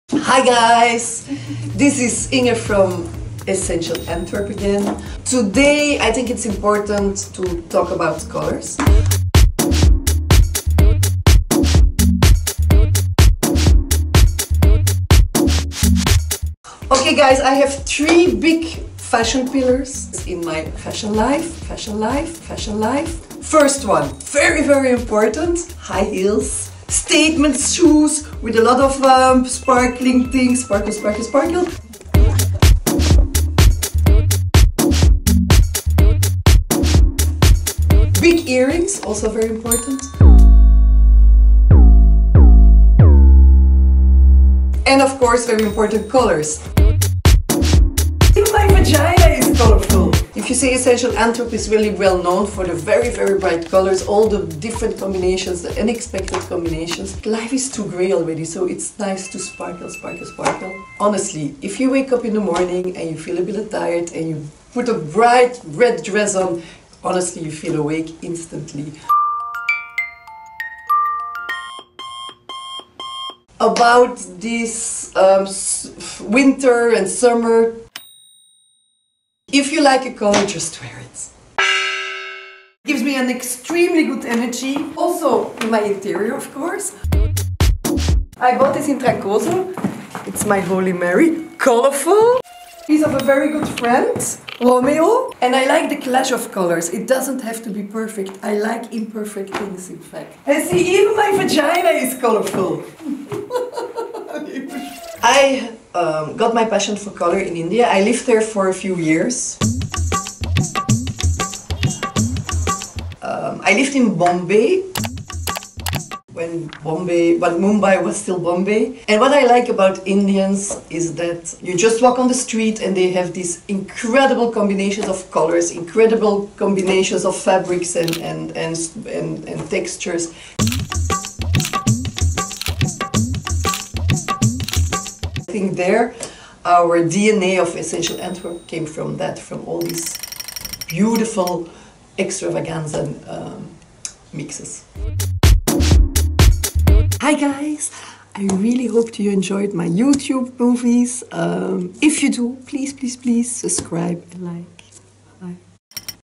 Color fever | ESSENTIEL ANTWERP
High heels, big earrings, and color. For Inge Onsea, our co-founder and creative director, those are the 3 fashion pillars. Watch her second YouTube video now to learn more about her life in India, and her colorful fashion and interior style!